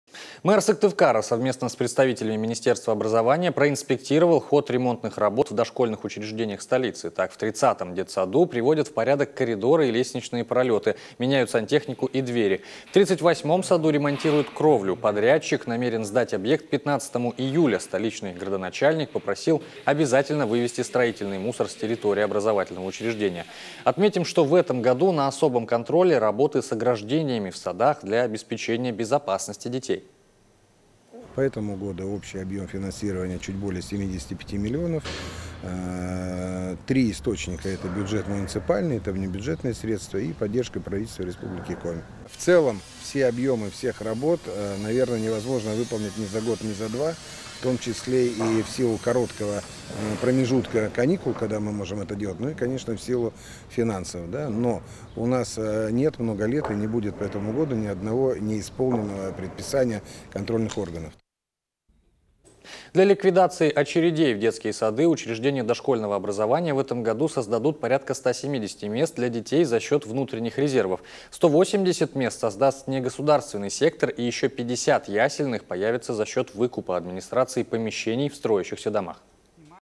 Ремонт садиков
Мэр Сыктывкара совместно с представителями министерства образования проинспектировал ход ремонтных работ в  дошкольных учреждениях столицы. Так, в 30-ом детсаду приводят в порядок коридоры и лестничные пролёты, меняют сантехнику и двери. В 38-ом саду ремонтируют кровлю. Подрядчик намерен сдать объект к 15 июля. столичный градоначальник попросил обязательно вывезти строительный мусор с территории образовательного учреждения. Отметим, что в этом году на особом контроле - работы с ограждениями в садах для обеспечения безопасности детей.